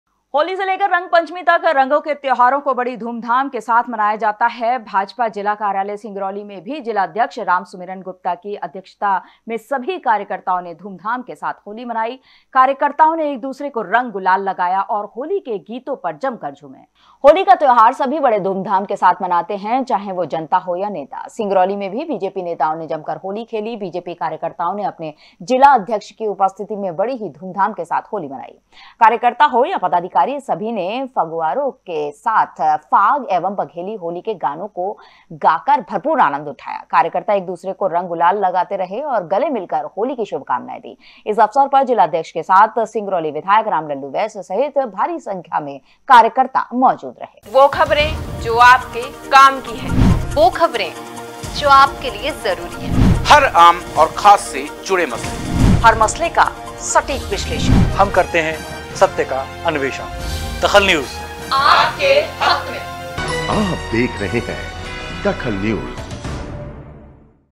DAKHAL NEWS धूम धाम से भाजपा कार्यकर्ताओं ने मनाई होली
धूम धाम से भाजपा कार्यकर्ताओं  ने मनाई होली
फाग के गीतों को गाकर महफिल में रंग जमाया